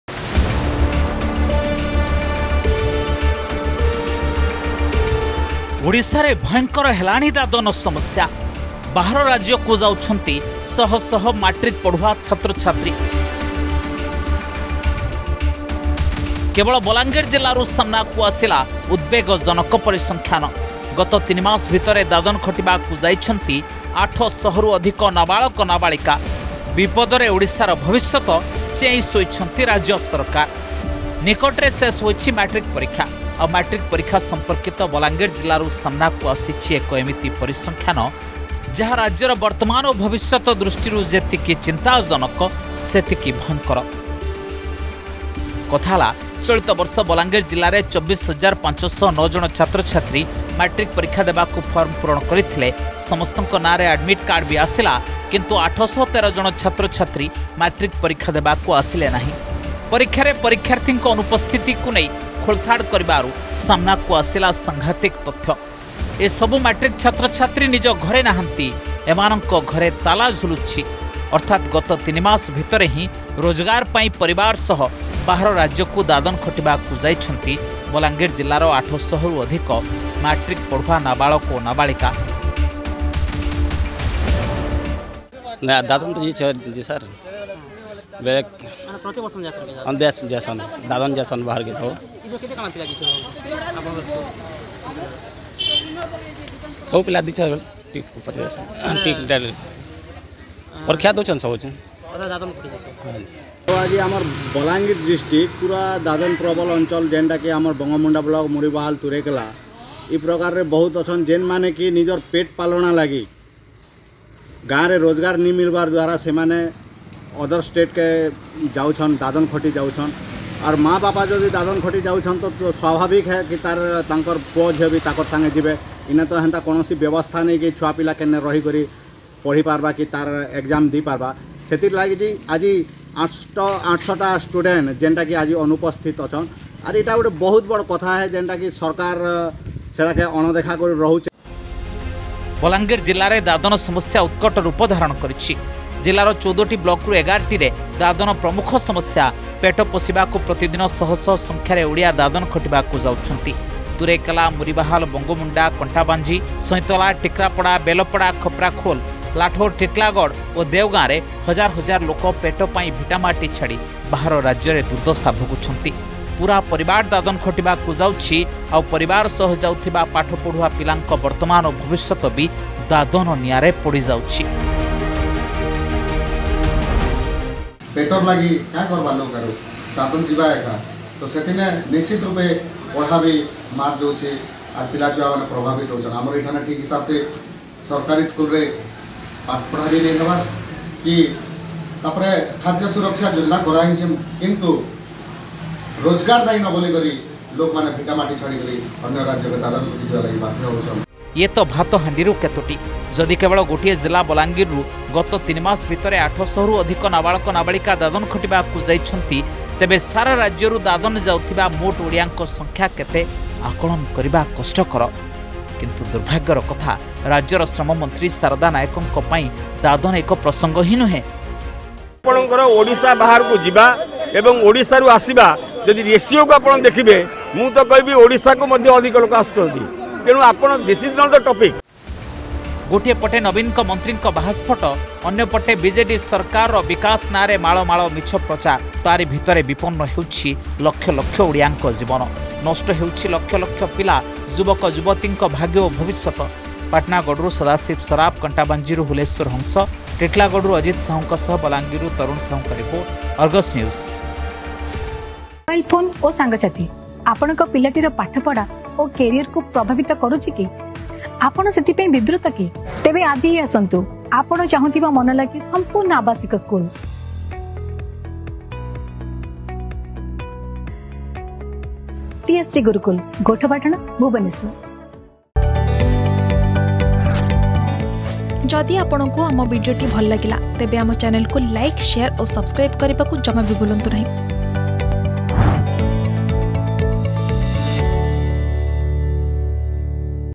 ଓଡ଼ିଶାରେ ଭୟଙ୍କର ହେଲାଣି ଦାଦନ ସମସ୍ୟା || Migrant Workers || Balangir
ଓଡ଼ିଶାରେ ଭୟଙ୍କର ହେଲାଣି ଦାଦନ ସମସ୍ୟା । ବାହାର ରାଜ୍ୟକୁ ଯାଉଛନ୍ତି ଶହ ଶହ ମାଟ୍ରିକ୍ ପଢୁଆ ଛାତ୍ରଛାତ୍ରୀ । କେବଳ ବଲାଙ୍ଗିର ଜିଲ୍ଲାରୁ ସାମ୍ନାକୁ ଆସିଲା ଉଦବେଗଜନକ ପରିସଂଖ୍ୟାନ। ବିପଦରେ ଓଡ଼ିଶାର ଭବିଷ୍ୟତ, ଚେଇଁ ଶୋଇଛନ୍ତି ରାଜ୍ୟ ସରକାର ।